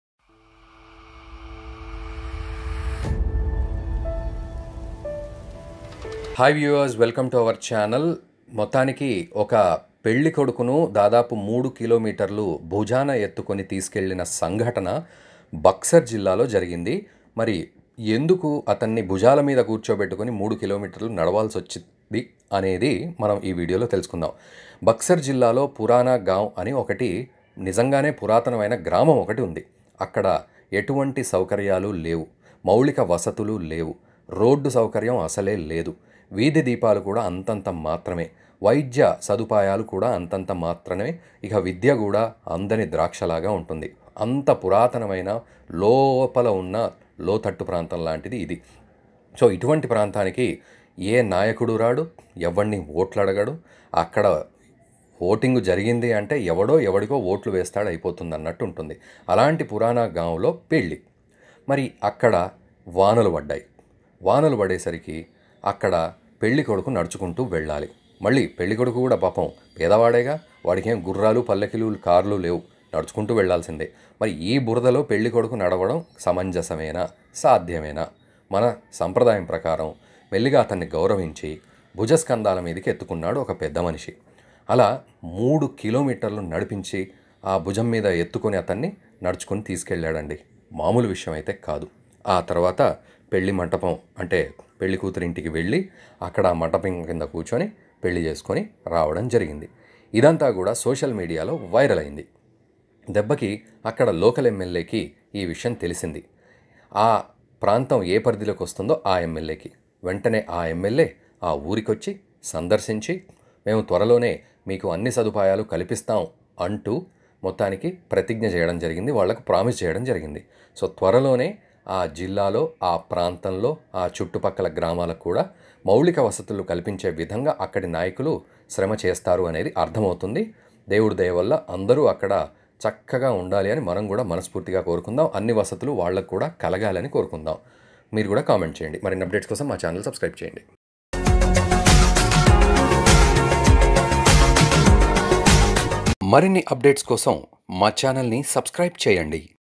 పెళ్లి చేసుకున్న వెంటనే భుజాల మీద కూర్చున్న పెళ్లి కొడుకు.! ఎందుకో కారణం తెలిస్తే షాక్ అవుతారు
Watch: పెళ్లి చేసుకున్న వెంటనే భుజాల మీద కూర్చున్న పెళ్లి కొడుకు.! ఎందుకో కారణం తెలిస్తే షాక్ అవుతారు.